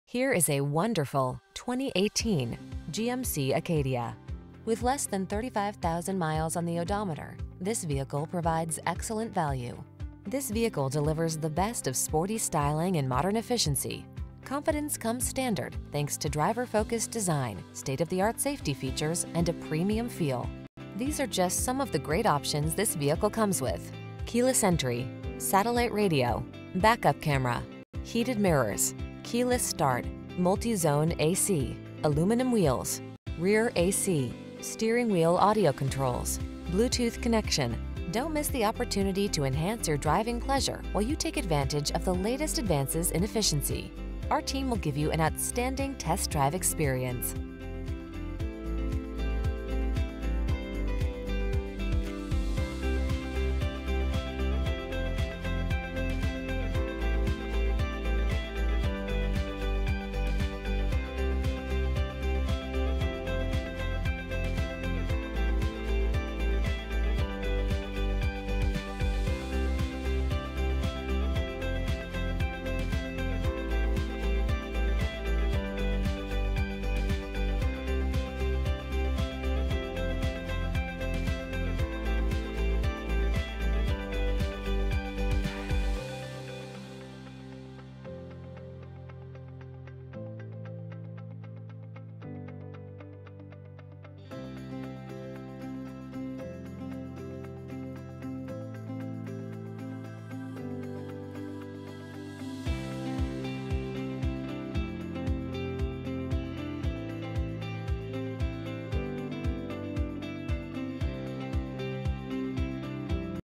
2018 GMC Acadia Worthington, Columbus, Westerville, Powell, Delaware XX1T181082B
Ebony Twilight Metallic Used 2018 GMC Acadia available in Worthington, Ohio at Mark Wahlberg Chevrolet of Worthington. Servicing the Westerville, Powell, Delaware area.

700 E. Dublin-Granville Road

CARFAX One-Owner. Clean CARFAX. Priced below KBB Fair Purchase Price!FWD Jet Black Cloth.Ebony Twilight Metallic 2018 GMC Acadia SLE-1 FWD 6-Speed Automatic 2.5L 4-Cylinder DGI DOHC VVT 21/26 City/Highway MPGOdometer is 1965 miles below market average! 21/26 City/Highway MPG